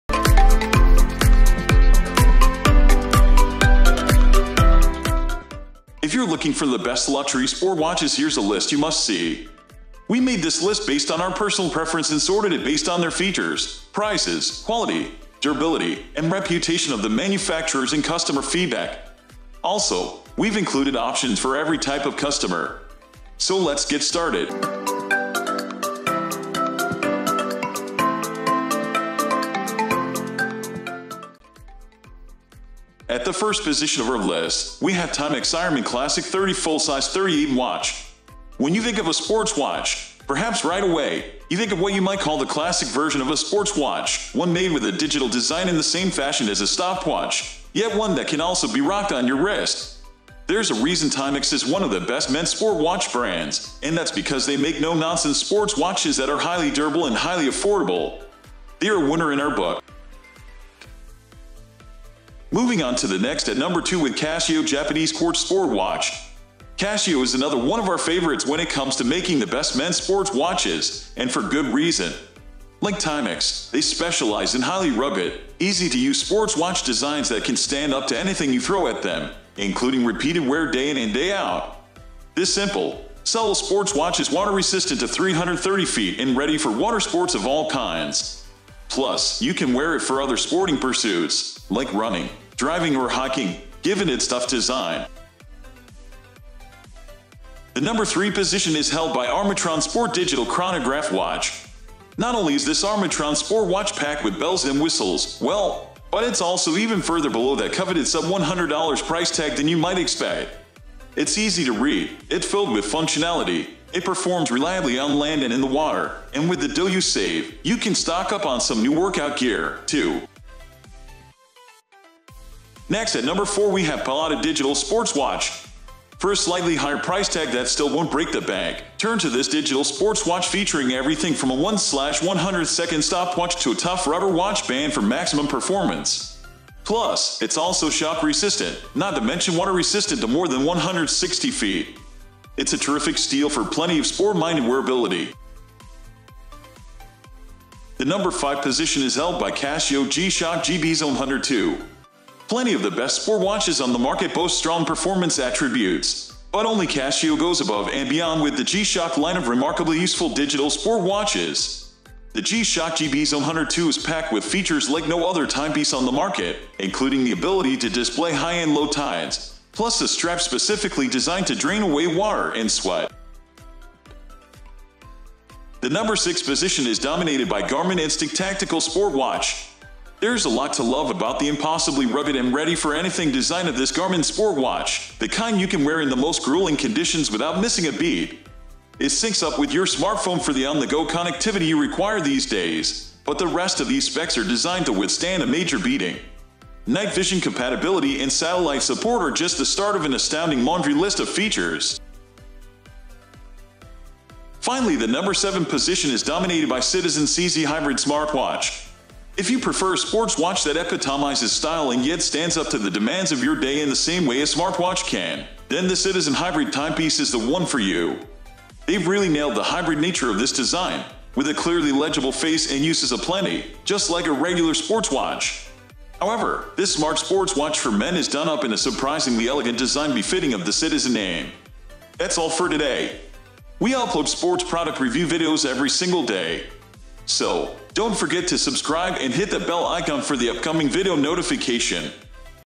Best Luxury Sport Watches in 2024 (Top 7 Picks)
Links to the Top 7 Best Luxury Sport Watches we listed in today's Luxury Sport Watch Review video & Buying Guide:

In this video, we have gathered the Luxury Sport Watch on the market in 2024. With all types of consumers in mind, we made this list through extensive research, pricing, quality, durability, brand reputation, and various other factors.

►Best Wedges for High Handicappers